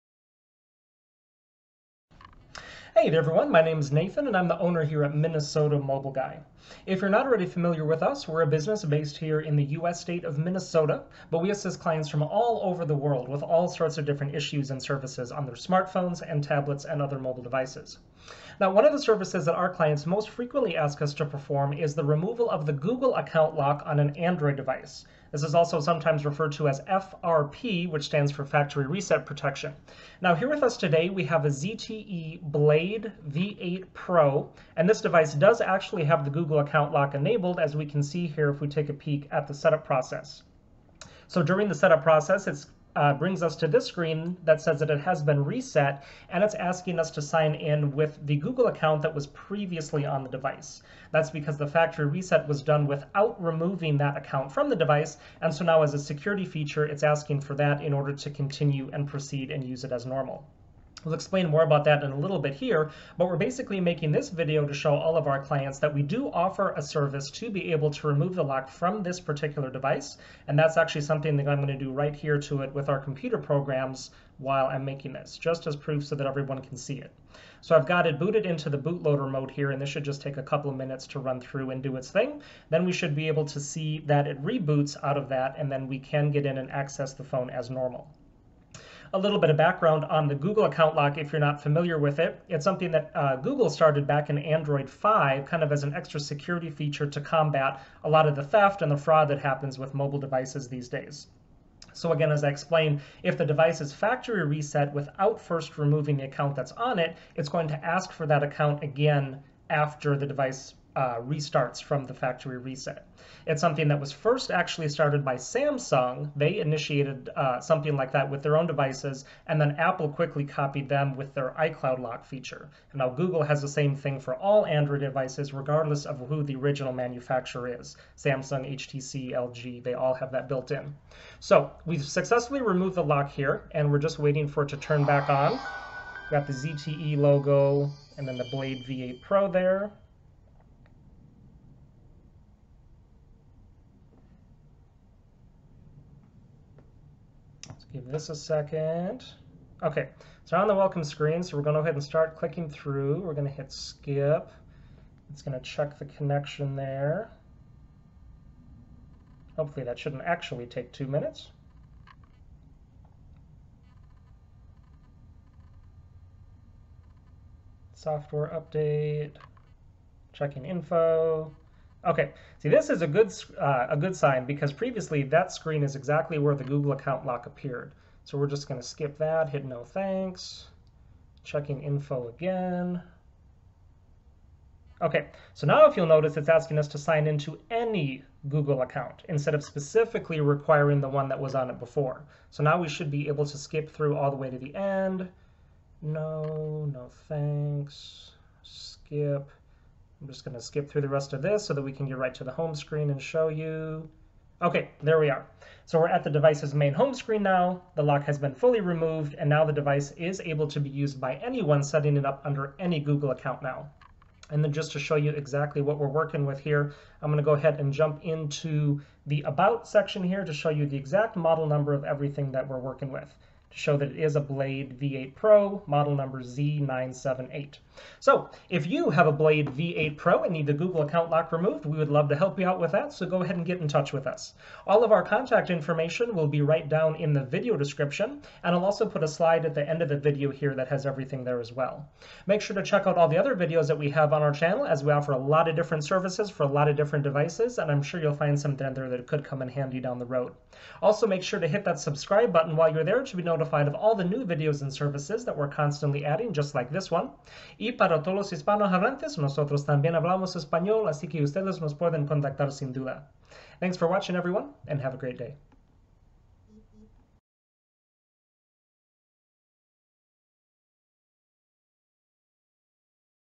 Disable Bypass Remove Google Account Lock FRP ZTE Blade V8 Pro!
Video showing a Google Account Lock successfully being bypassed/removed on a ZTE Blade V8 Pro (Z978). Google includes this security feature with all devices on Android 5.1 and up so that when a device is reset without first removing the Google account, the device is locked to the previous owner's account.